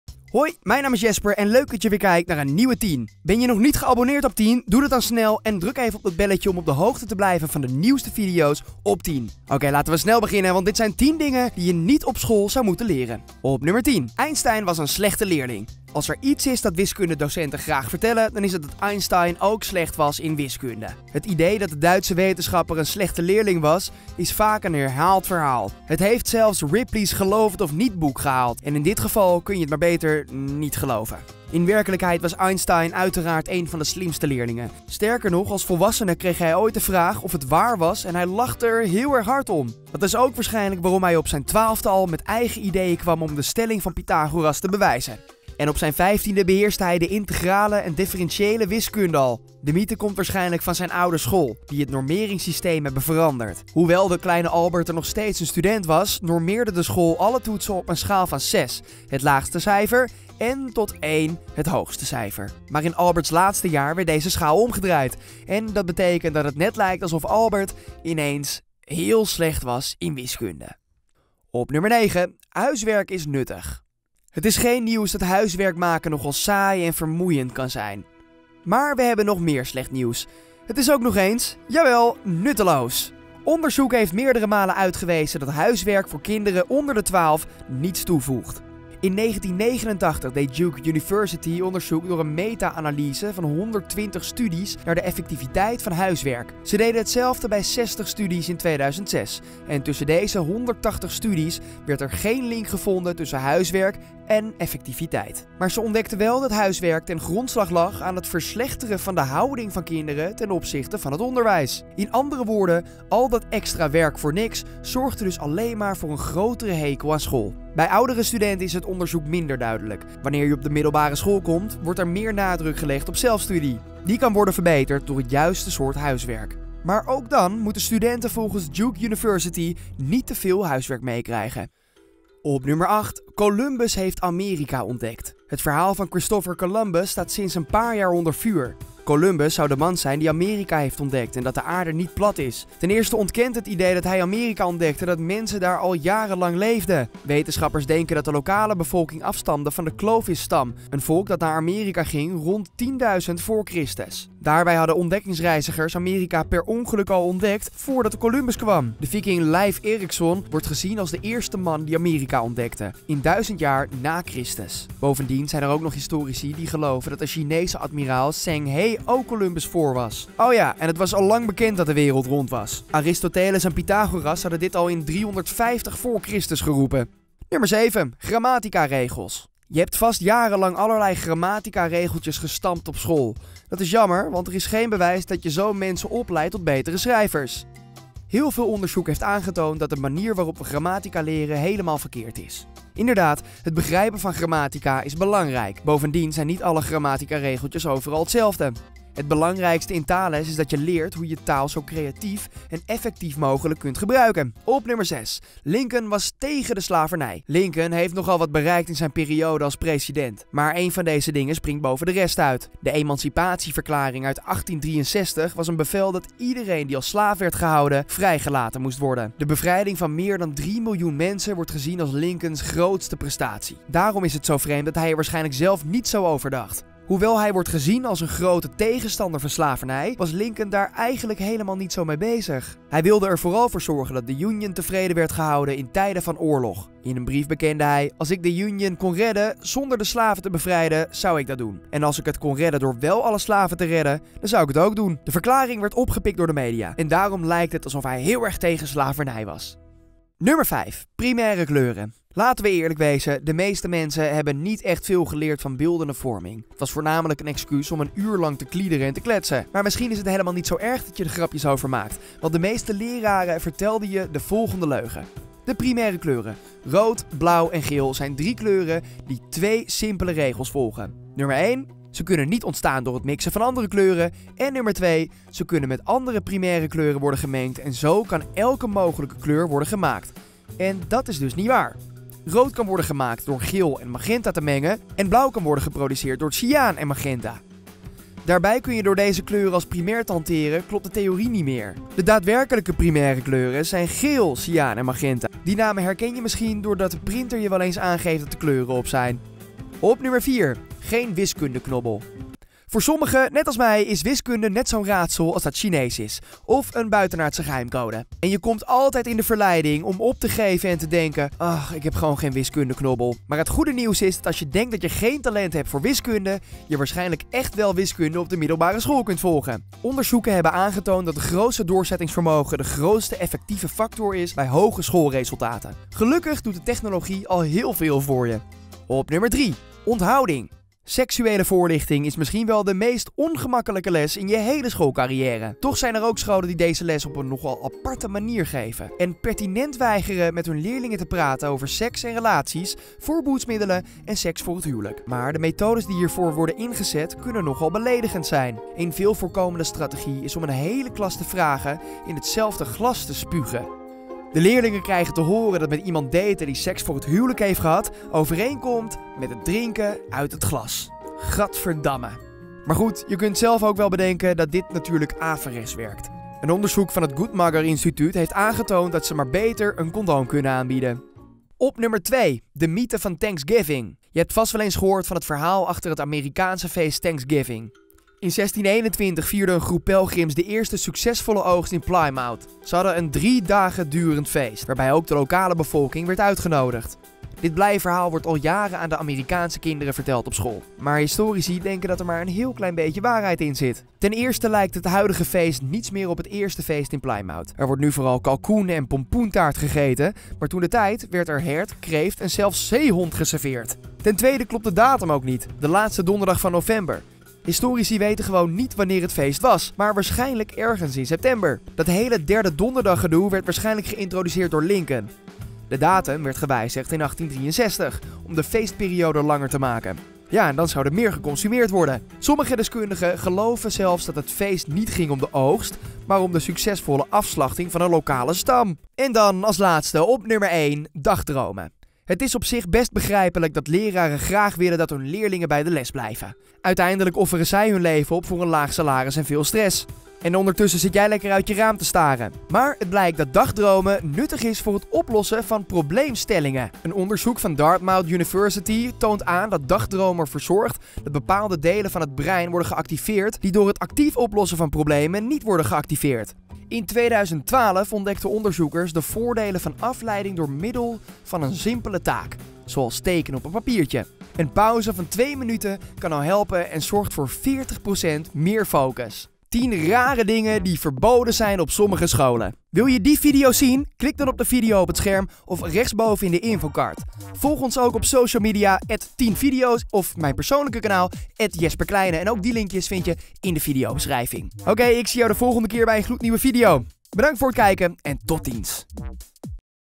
10 DINGEN DIE JE NIET OP SCHOOL ZOU MOETEN LEREN! - TIEN
⬇Check hieronder voor meer info!⬇

Vandaag behandelen we TIEN dingen die school jou niet goed geleerd heeft! Welke kwam voor jou het meest als een verrassing?

Voice-over: Jesper Kleynen
Vertaling: Nienke Slobbe

Het kanaal TIEN werkt samen met AllTime10s om de Engelse video's naar het Nederlandse publiek te brengen!